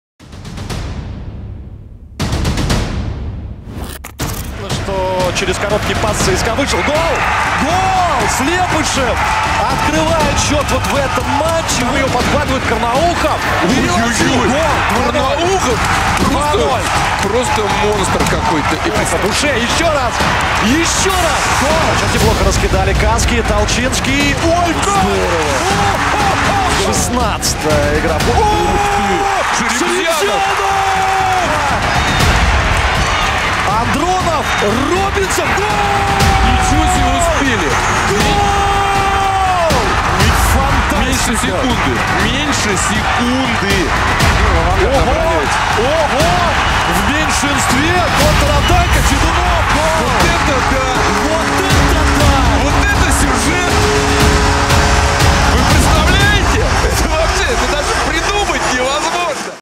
Кубок Гагарина за 60 секунд — 24 апреля 2021
Постараемся пройтись по событиям вчерашней безумной игры за 60 секунд.
Нужен полный обзор на 6.5 минут? Пожалуйста

Пропустив две шайбы и не реализовав в промежутке между ними штрафной бросок, хоккеисты «Авангарда» нашли в себе силы всё же переломить ход матча.

Плей-офф. День 41. Отыгрыш «Авангарда», сотое очко Толчинского